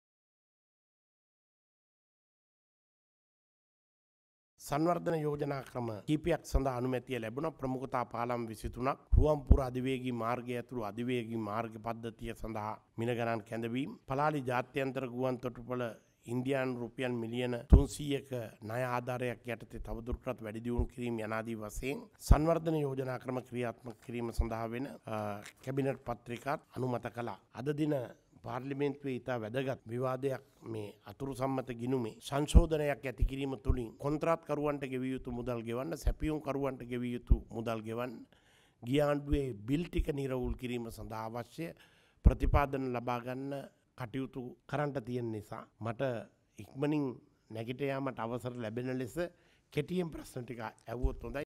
2020.02.20 - අමාත්‍ය මණ්ඩල තීරණ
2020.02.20 - අමාත්‍ය මණ්ඩල තීරණ  - පලාලිහි පිහිටි යාපනය ජාත්‍යන්තර ගුවන්තොටුපොළ වැඩිදියුණු කිරීම සඳහා ඉන්දියානු රජය සමඟ අවබෝධතා ගිවිසුම අත්සන් කිරීම